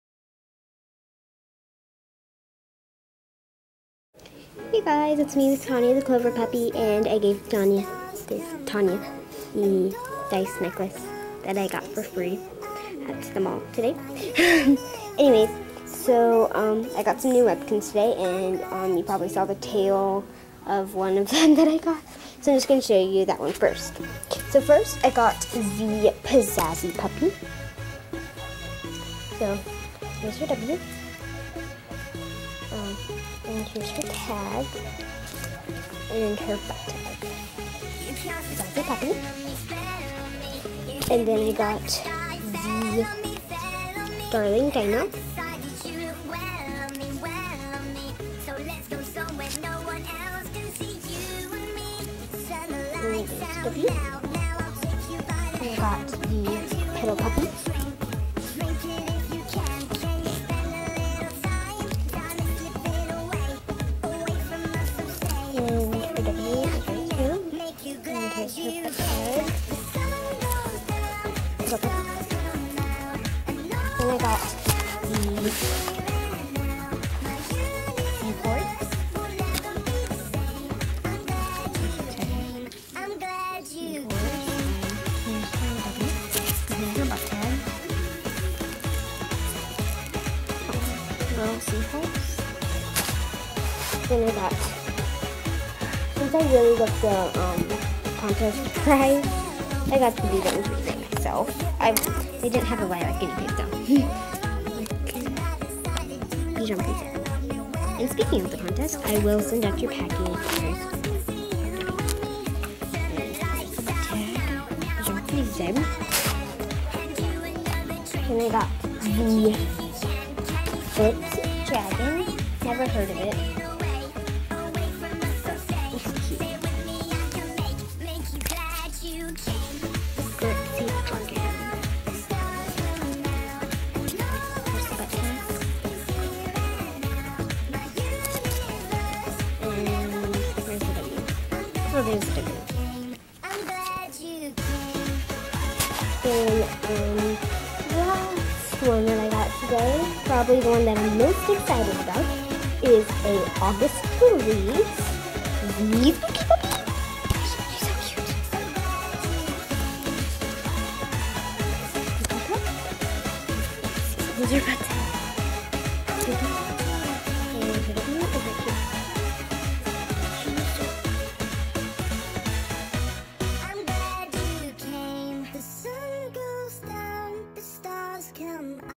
New Webkinz!
I Got New Ones Today (IN VEGAS BABY!)